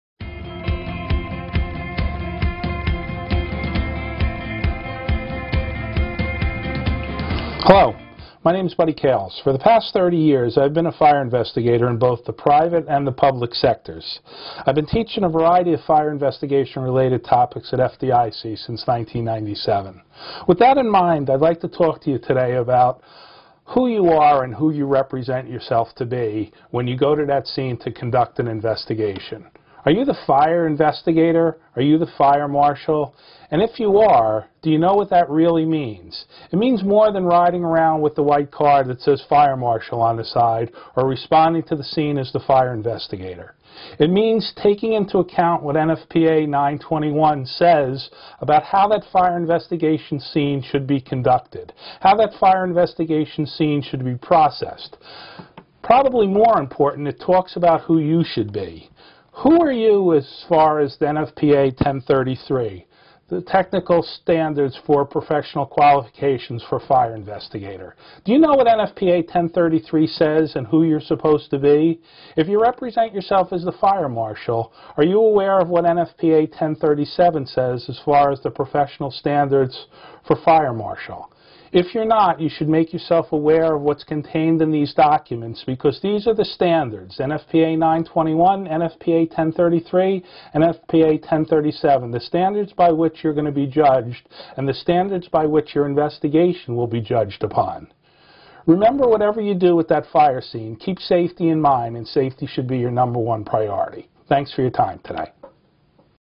Who Are You and Who Do You Represent: The Scene of a Fire Investigation
Buddy Cales discusses the thought process upon the scene of a fire.